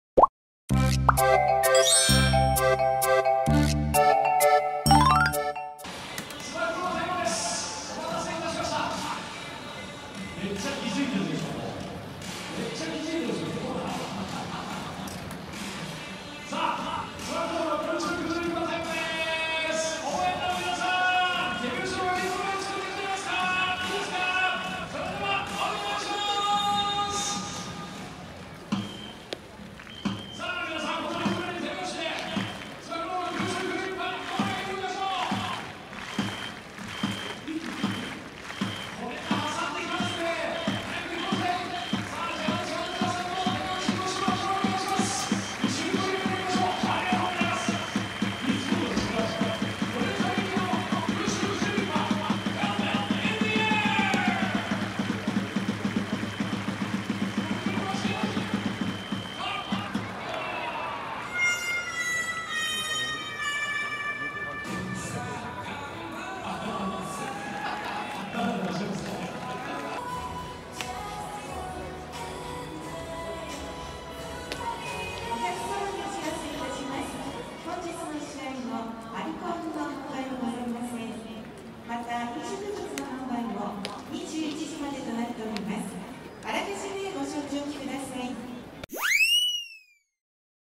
くるりんぱ前にアイーンをしながらチャレンジ！つば九郎空中くるりんぱ | 2021年10月15日巨人戦（神宮球場）
つば九郎の空中くるりんぱです。

2021年10月15日 巨人戦（神宮球場）